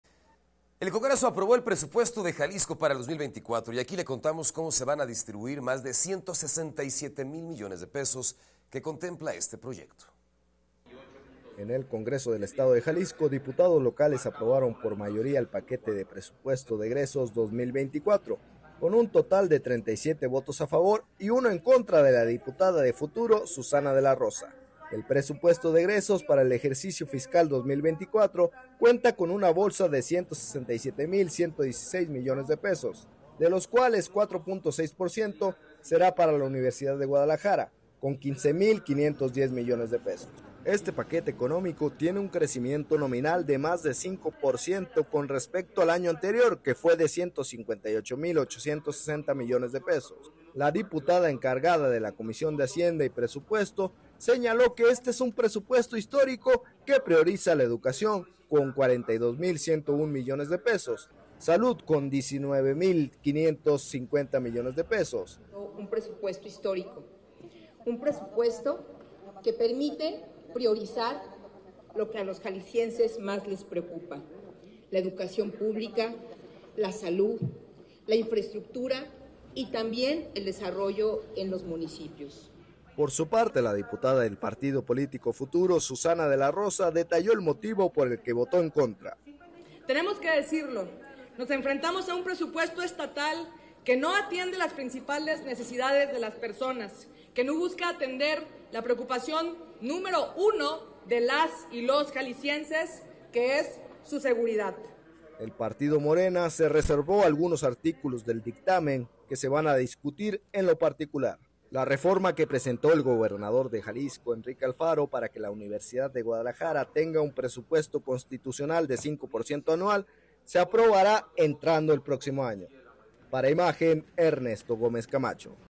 Así se distribuirá el presupuesto de egresos aprobado por el Congreso | Imagen GDL Ricardo Camarena
El Congreso aprobó el presupuesto de Jalisco para el 2024, en el cual se van a distribuir más de 167 mil millones de pesos.